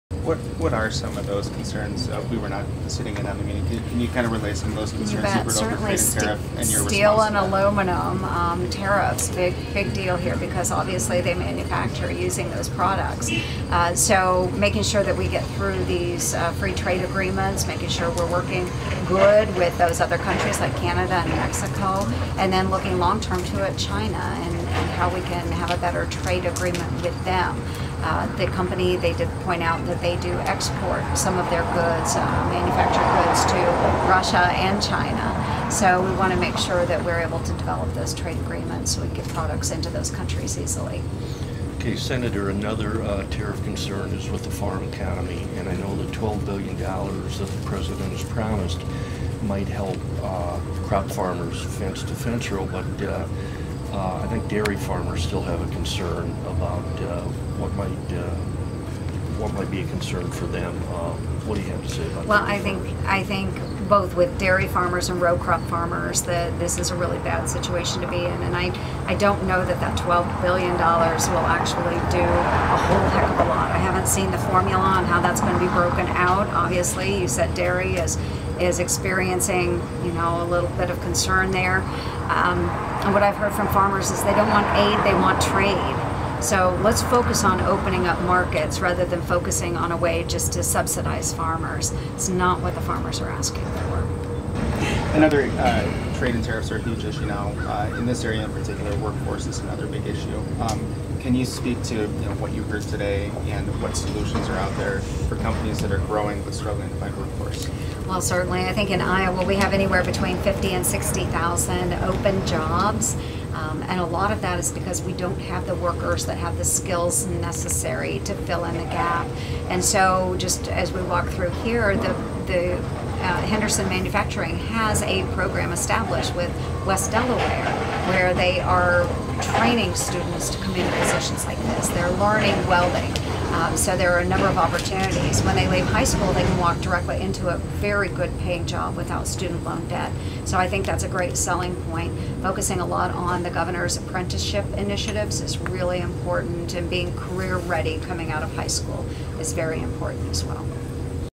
U.S. Sen. Ernst discusses trade tariffs
U.S. Sen. Joni Ernst, R-Iowa, discusses trade tariffs after touring Henderson Manufacturing in Manchester, Iowa on Wednesday.